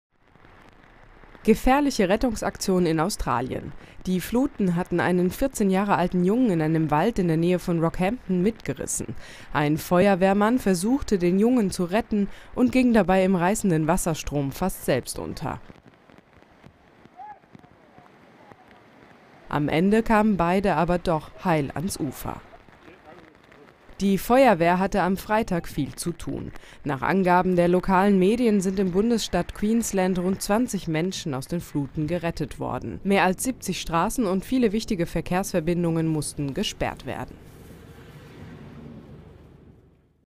Überschwemmung in Australien: Dramatische Rettungsaktion
Mit letzter Kraft hatte sich der 14 Jahre alte Junge in den Fluten an einem Baum festgehalten. Ein Feuerwehrmann holte den Jungen aus dem Wasser und ging dabei fast selbst unter. Starke Regenfälle haben weite Teile von Queensland überschwemmt.